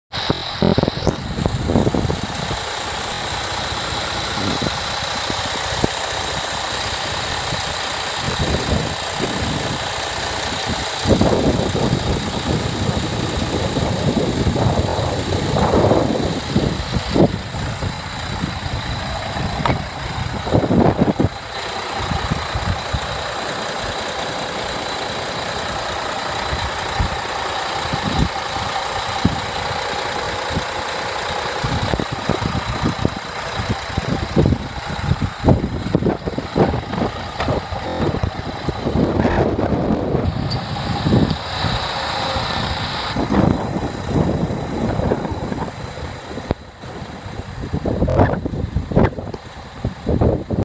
2012 International ProStar+ Semi | For Sale | Online Auction at Repocast.com
2012 International ProStar+ Semi | For Sale | Online Auction at Repocast.com

Poly quarter fenders, APU turns over but won’t start, Check Engine Dash Warning, scattered exterior paint chips, windshield chipped, interior is overall clean, Runs and Drives